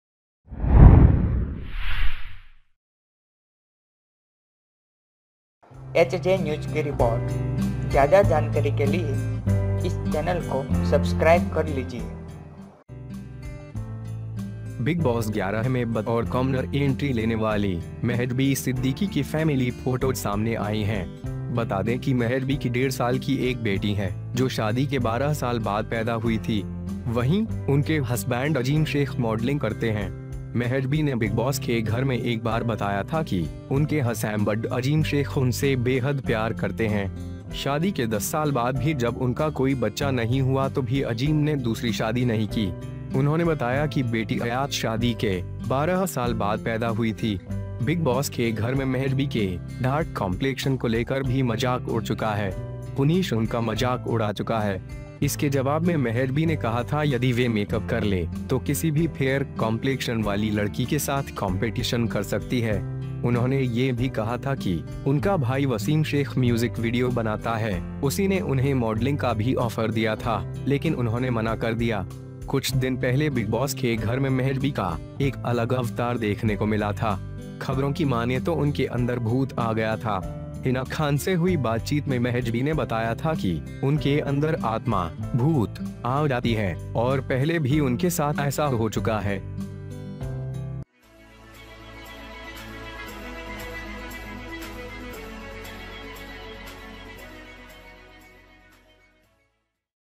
शादी के 12 साल बाद मां बनी थी 'बिग बॉस' की ये कंटेस्टेंट
Family photos of Mrs Siddiqui, who took a common entry as Big Boss 11, have come out. Let me tell that there is a daughter of one and a half year old mother who was born after 12 years of marriage. At the same time, their husbands Azim Sheikh modeling do. Mahajan had told once in the house of Big Boss that his Husambam Azim Sheikh loves him very much. Even after 10 years of marriage, when he had no child, Azim did not get another marriage. He told that daughter Ayy was born after 12 years of marriage

In the house of Big Boss, there is a joke about the dark complexion of Mihajby. Punish has got rid of them. In response, Mahjibi had said that if they do make-up, they can compete with a girl with any fair complexion. He had also said that his brother, Wasim Sheikh creates music videos, he also offered them modeling, but he refused.
A few days ago, a different incarnation of Mahabby was found in Big Boss's house. If the news were to be believed then the ghost came in them. In a conversation with Hina Khan, Mahjibi had told that the soul (ghosts) came in them and that had happened to them before.

HJ NEWS !!